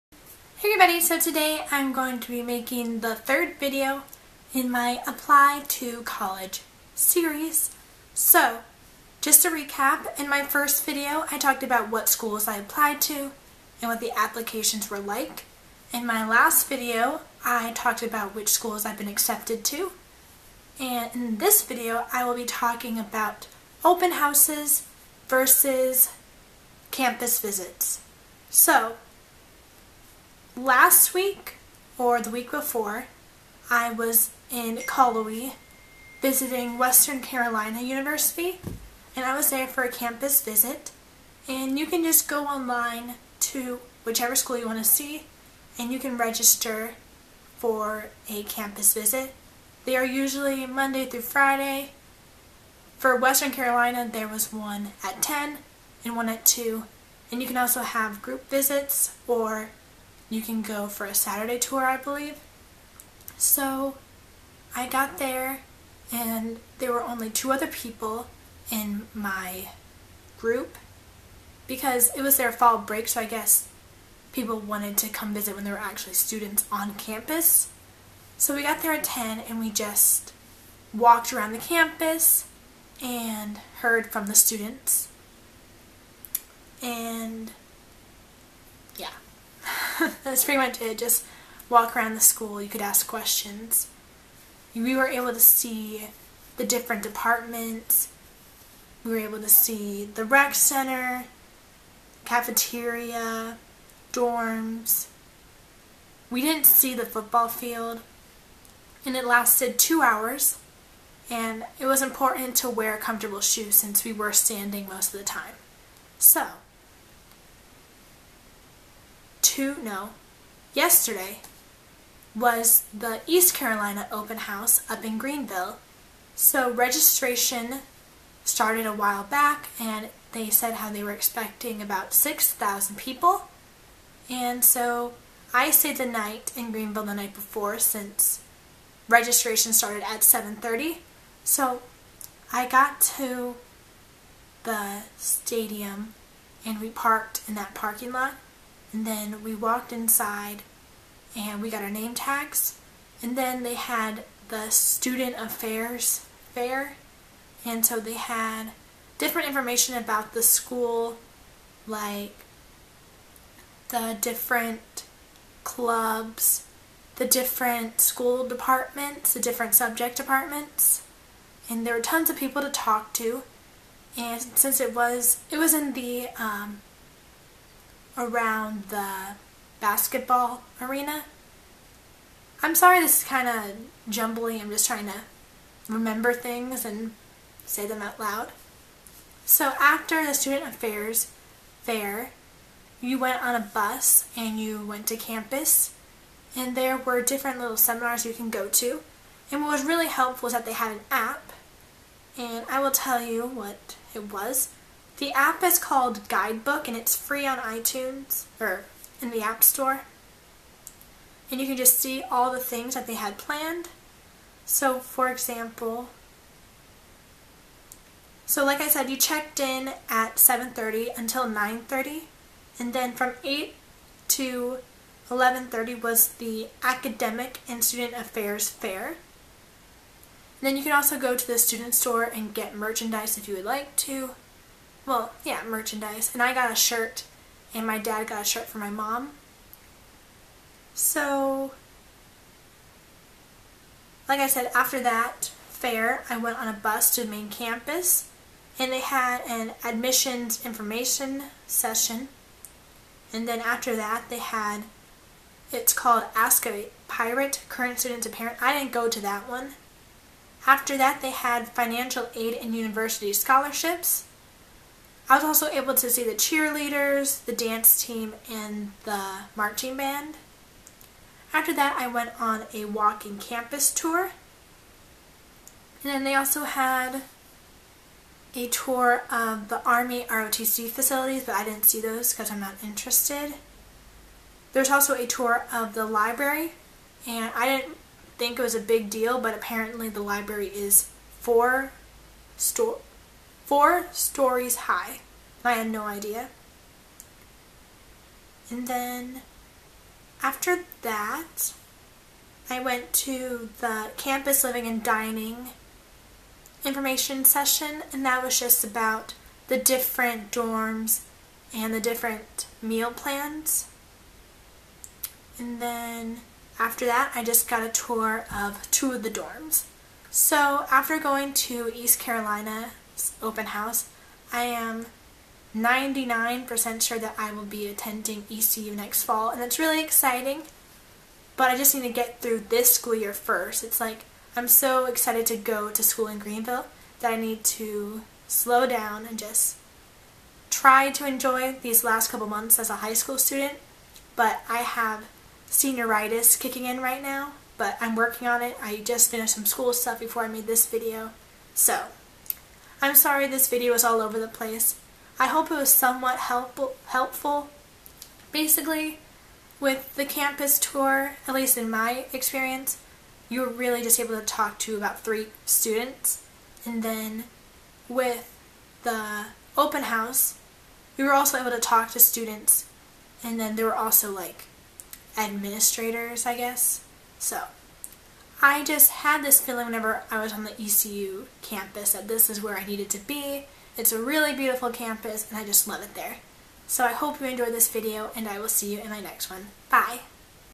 Applying to College | Episode 3 | Campus Visits vs. Open House
Hey everybody!
College applications for fall 2016 are officially open! In this series I'll talk about my experiences & give tips & tricks!